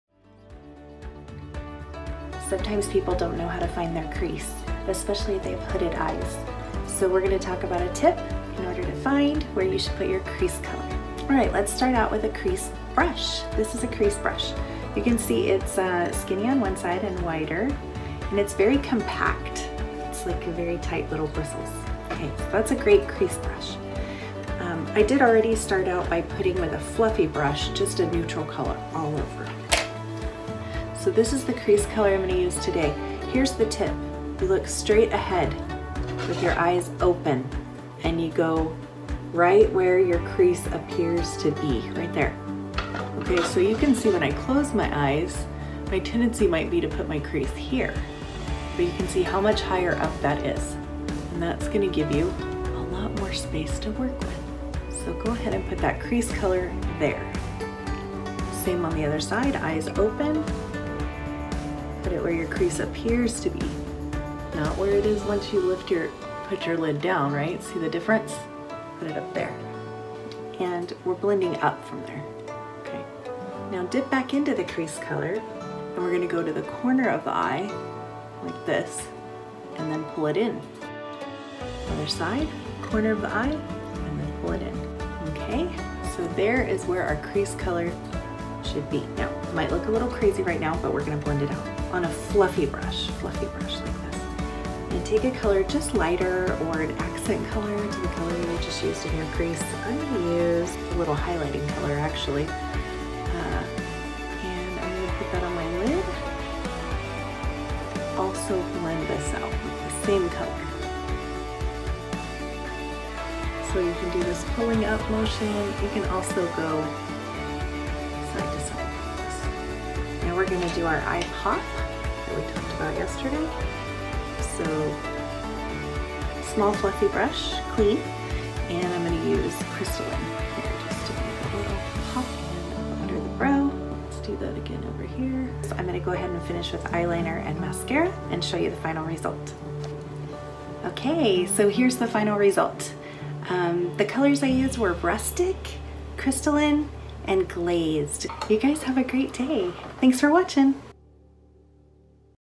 Where Is My Crease? Eye Makeup Tutorial
Are you putting your crease color too high, too low or have no idea where to place it? Let's fix that with a simple tip! Do your eye makeup like a pro!